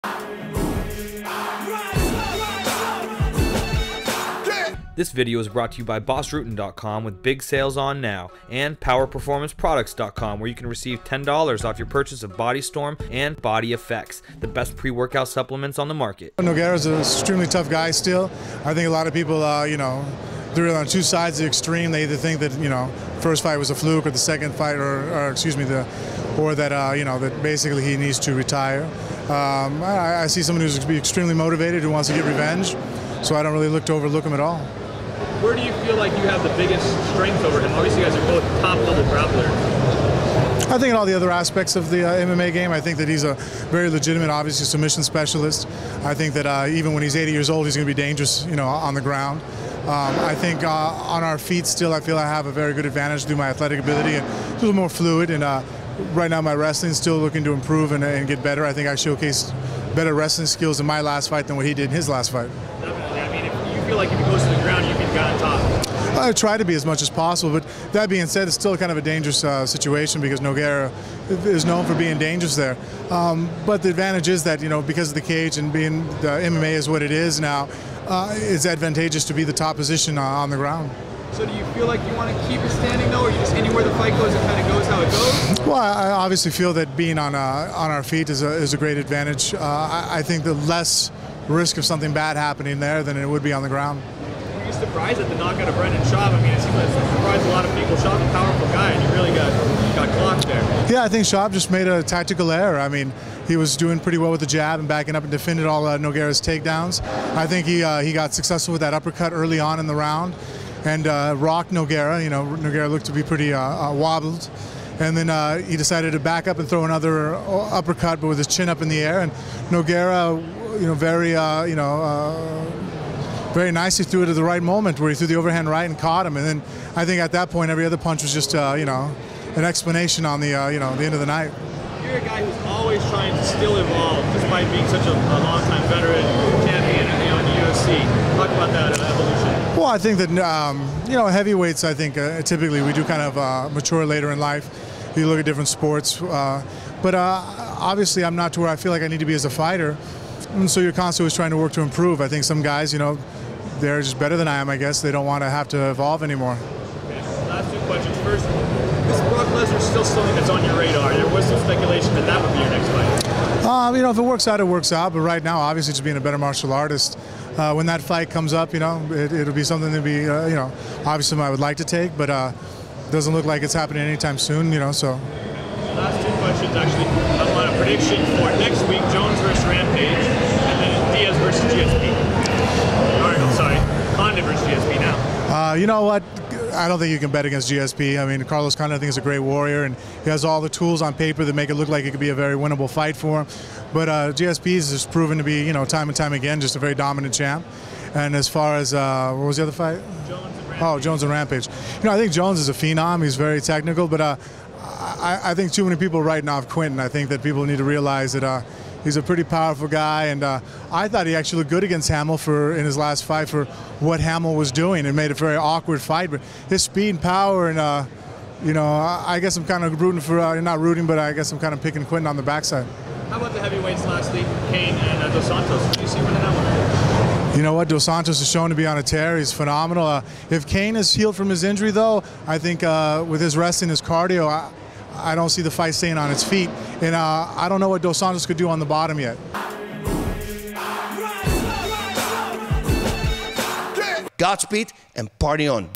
Frank Mir on Nogueira rematch at UFC 140, Cain vs Dos Santos, GSP vs Condit at UFC 137 & Lesnar
Spencer Lazara caught up with Frank Mir at the Olympia Convention in September.  Mir gave his thoughts on his rematch with Antonio Rodrigo Nogueira at UFC 140.  He also breaks down Cain Velasquez vs Junior Dos Santos, wrongly picks Rampage in an upset over Jon Jones and takes Georges St. Pierre over Carlos Condit at UFC 139.

He also mentions that a third fight with Brock Lesnar would be something he'd like do in the future and talks about his constant evolution as a fighter/martial artist.